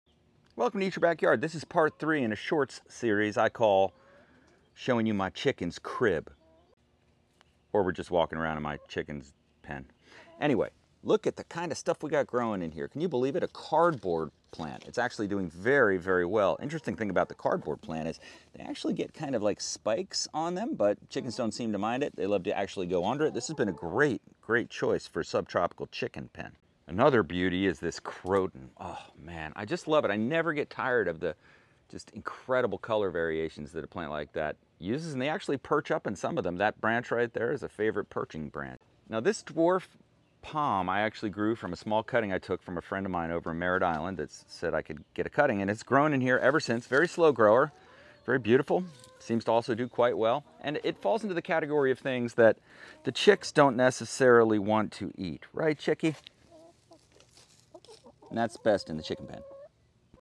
Part 3 - My Chicken Crib Tour, Cardboard Plants, Croton, Figs, Cherries and Sweet Chickies!
This is the third short in a deep dive of my chicken run.  Check it out, I hope you enjoy it.

Great Gardening Products